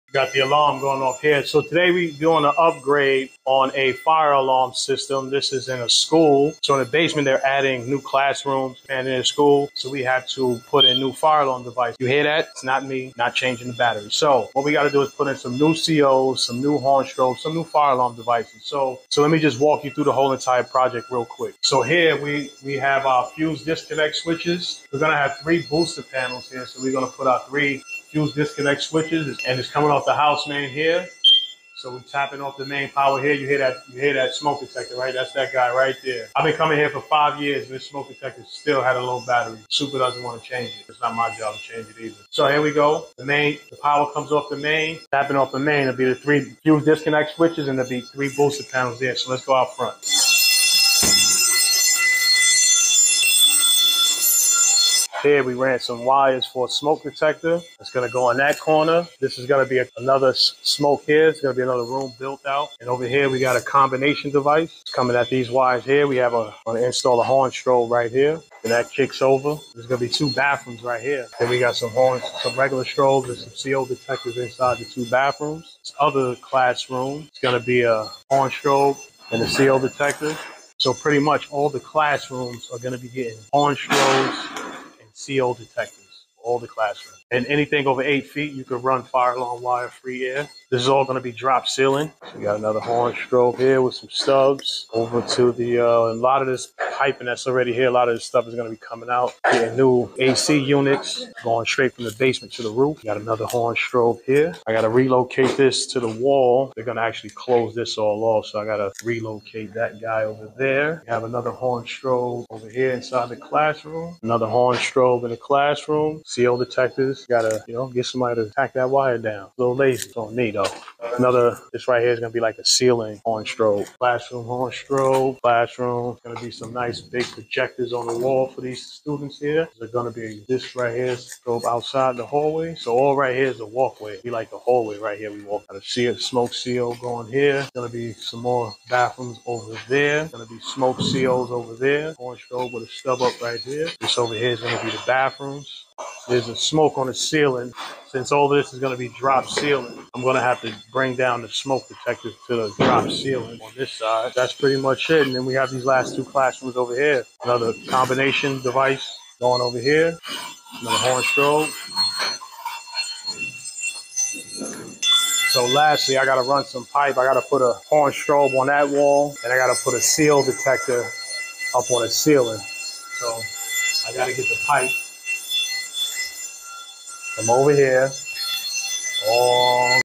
Testing School Fire Alarms (and Installing New Ones)
In this video I explains how we are upgrading the fire alarm system, adding new fire alarm devices such as CO detectors and horn strobes. I also show the location of the fuse disconnect switches and booster panels.